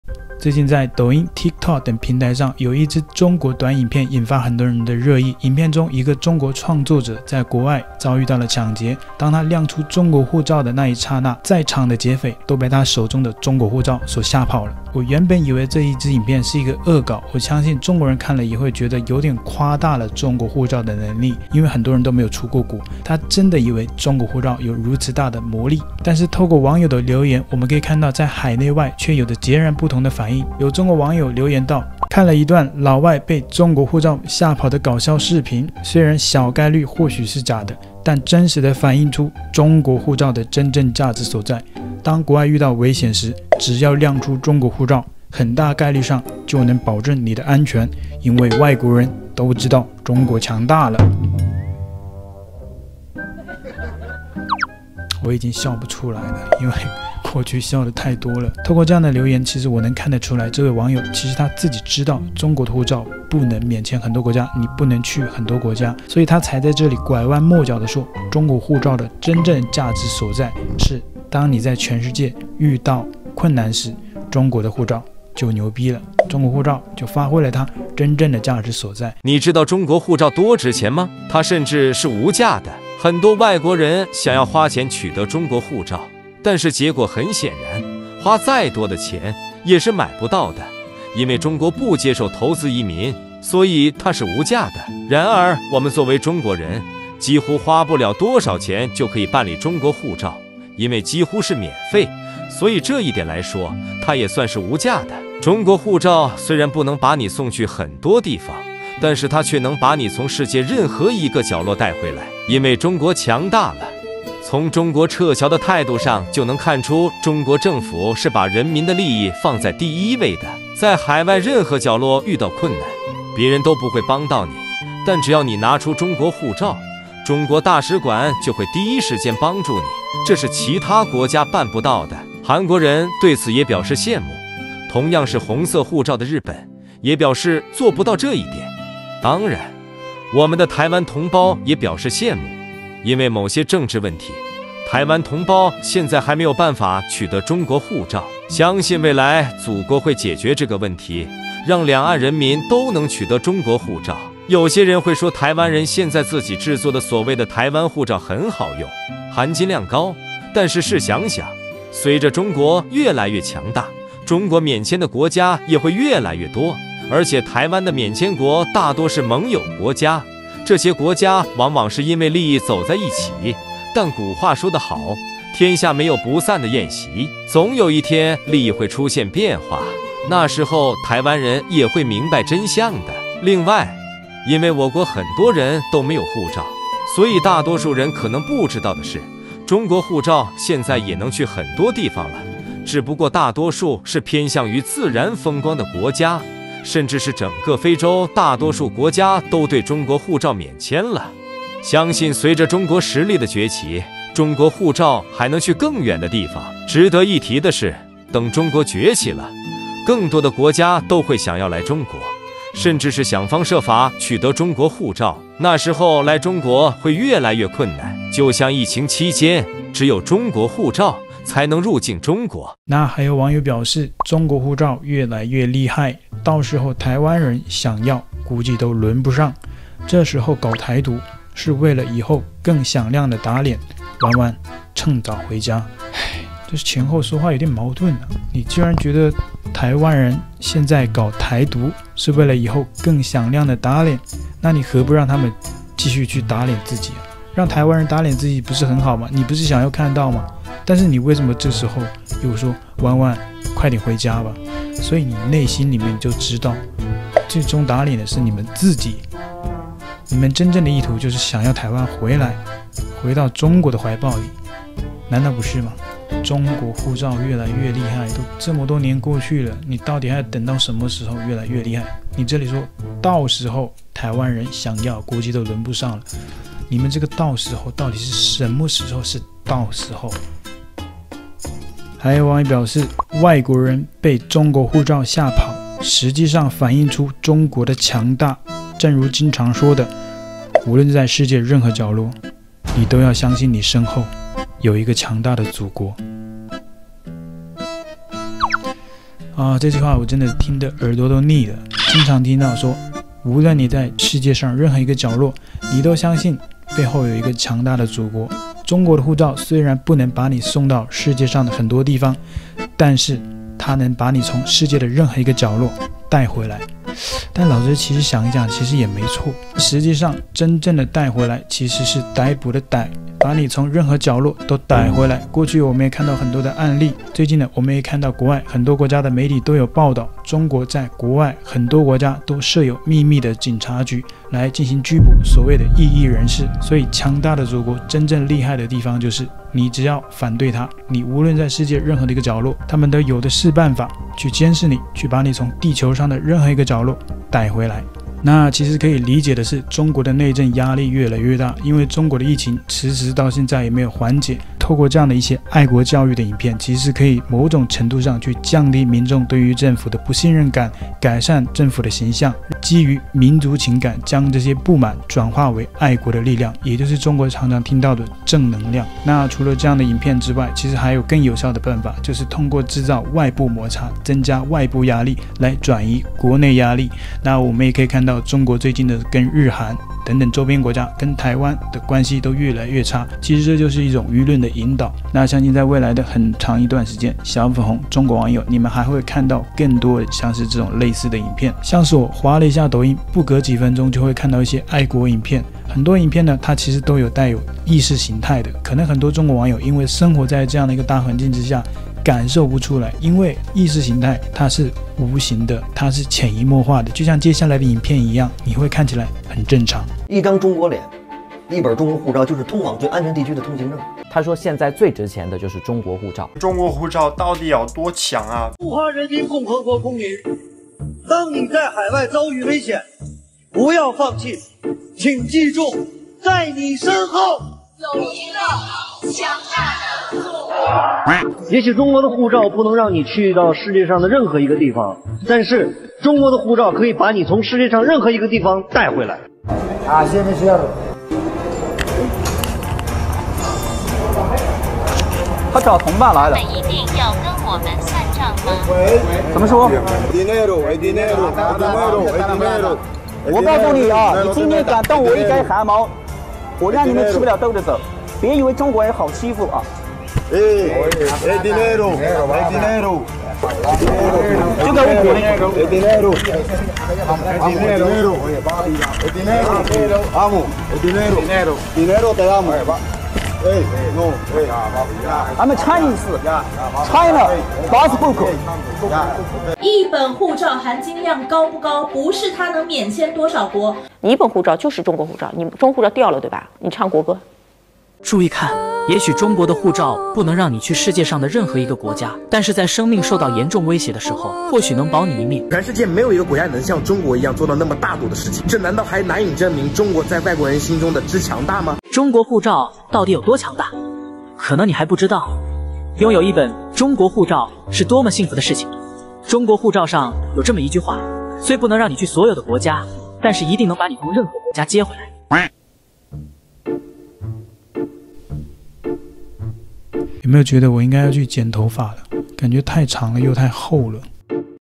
兩岸護照誰更強？大陸網友不服輸！她說中國護照這點台灣比不了！中國留學生海外遭搶劫 拿出中國護照嚇退劫匪！微博抖音網友直言「台灣人民其實也羨慕」⋯⋯
兩岸護照誰更強？大陸網友不服輸！她說中國護照這點台灣比不了！中國小粉紅海外遭打劫勒索 秀出中國護照嚇退土匪！愛國戰狼吳京曾經說過「無論你在海外遇到了怎樣的危險，請你記住，你的背後有一個強大的祖國」⋯⋯台灣人看完感到超傻眼！？